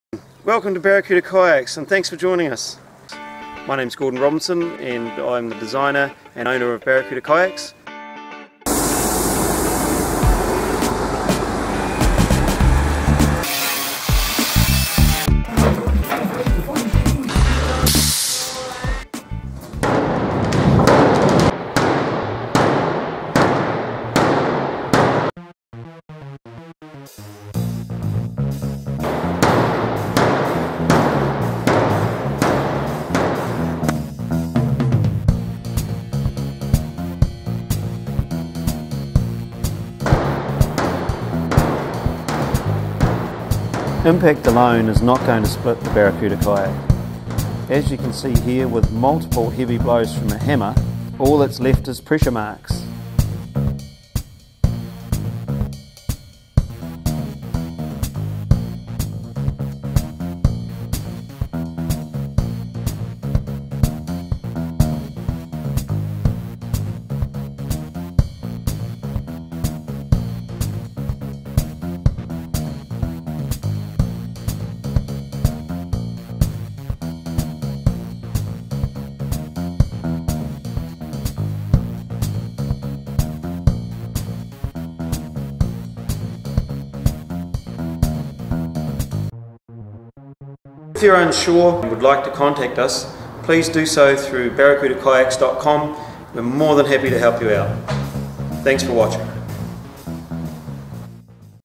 Barracuda Kayaks...Will it BREAK!? that is the question...watch and see
So you asked the question, and we have answered it... Lightweight does not mean Fragile... the misconception among some consumers is that our lightweight plastic makes the Barracuda kayaks fragile..For an example we show here in this hammer test, that is not true...The plastic we use is our own unique, homogenised, blend..meaning the UV stabilisers and strengthening agents are built into the Barracuda plastic (other vacuum formed brands use a capped ABS plastic) ..the Glue used to join our kayaks creates a chemical bond, effectively welding the pieces together... if in doubt please contact us and we would be happy to hear your thoughts.